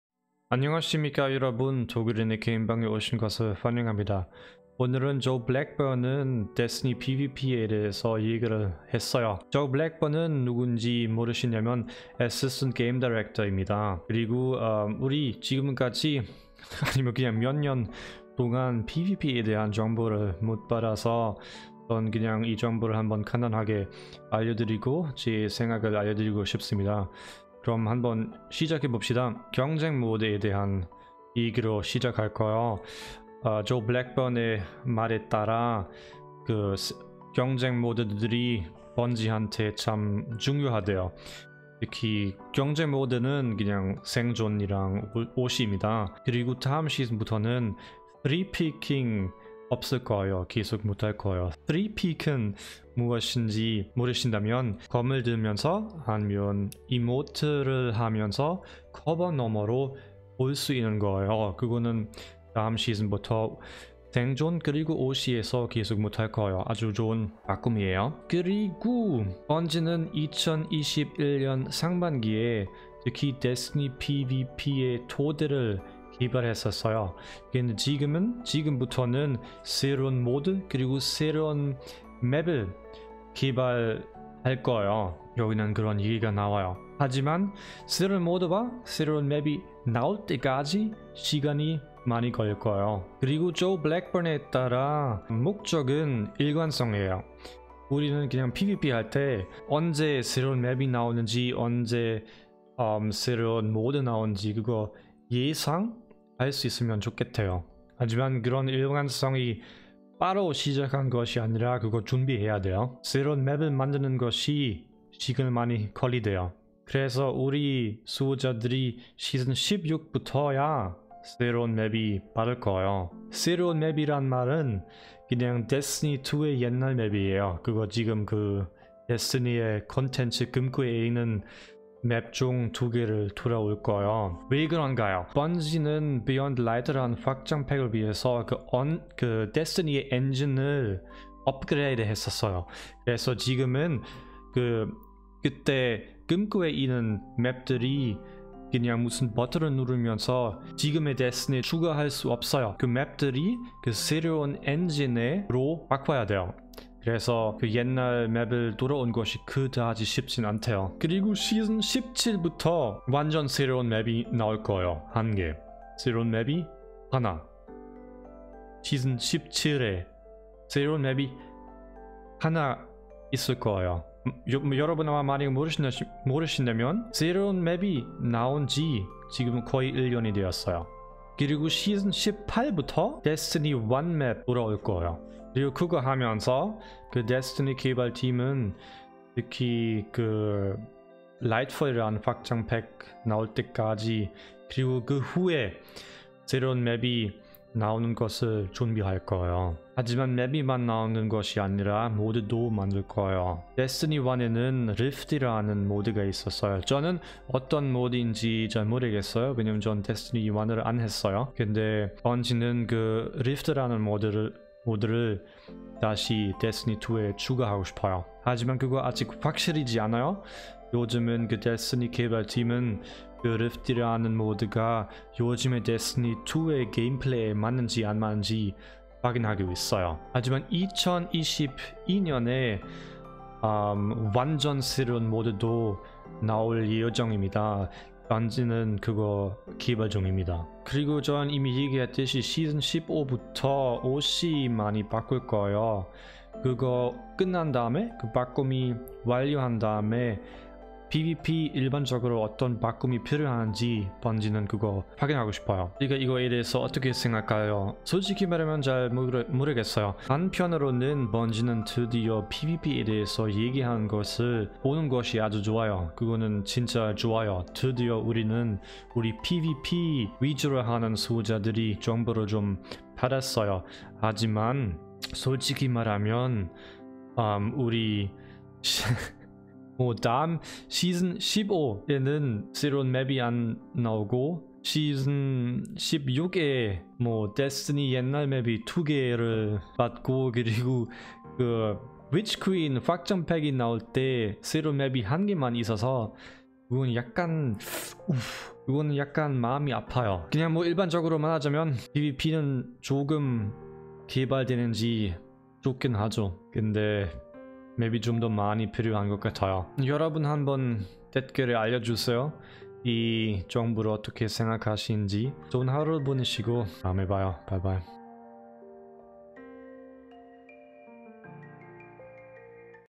시련의 장 (PVP) 정보가 나왔다. 맵과 모드가 온다 | 데스티니 가디언즈 융합의 시즌 뉴스
Joe Blackburn님은 데스티니의 PVP에 관한 정보를 우리한테 알려주셨어요. 이 영상에는 이 정보를 한국어로 해석해주고 제 생각을 알려드릴게요.